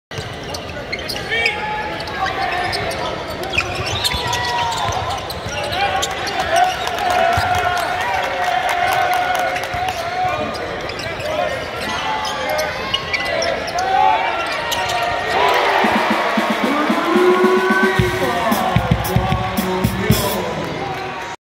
Juan Munoz First Triple Against Central Arkansas!
UH's Juan Munoz hits one of his 5 triples here!

Munoz had 19 pts. in the 95-76 win over Central Arkansas!

Game: UH Vs. Central Arkansas (12/3/23)
Non-conference Home Game #4